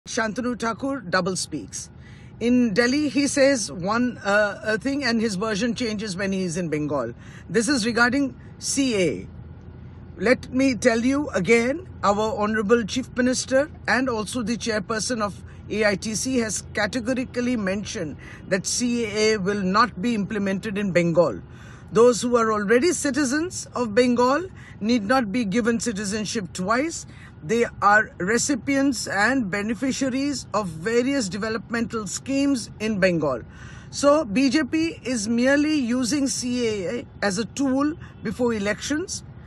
TMC's Shashi Panja Accuses BJP's Shantanu Thakur of Flip-Flopping on CAA
Shashi Panja, TMC leader, criticizes BJP's Shantanu Thakur for contradictory statements on CAA, asserting that Bengal won't implement it